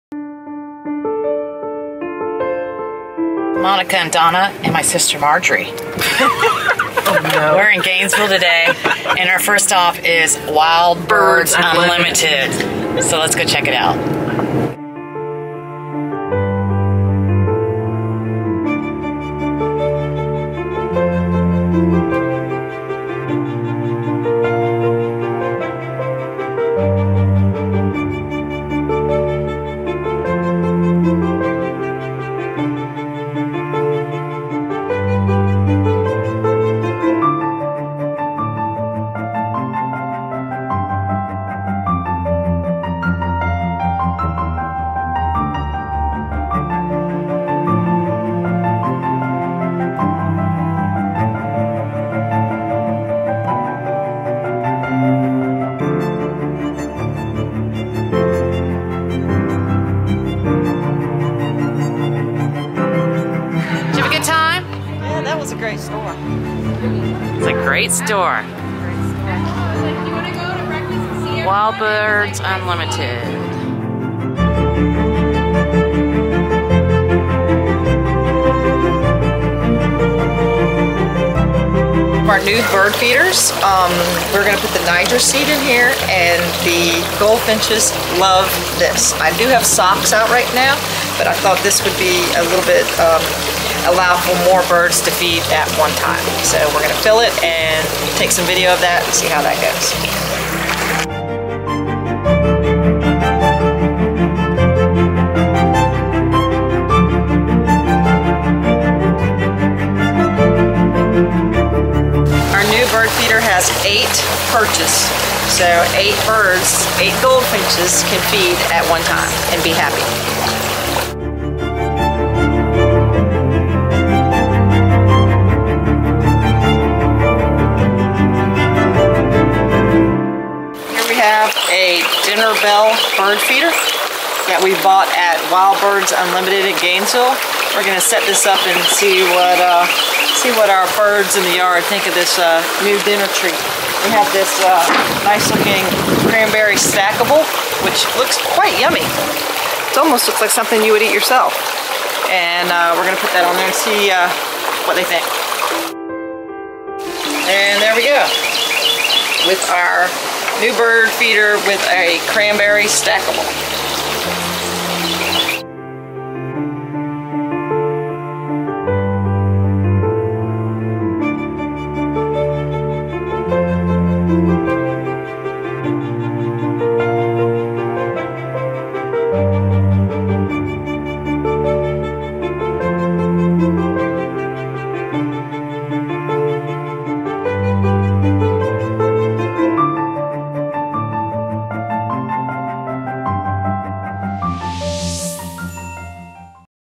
It’s a birds life at Casa Donica
Our trip to Wild Birds Unlimited and setup of our new bird feeders